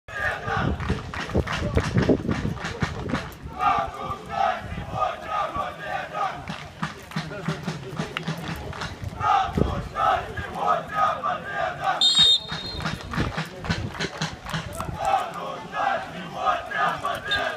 Фанати ФК "Енергія". Нова Каховка
фан сектор на стадіоні Енергія в Новій Каховці ( Херсонщина).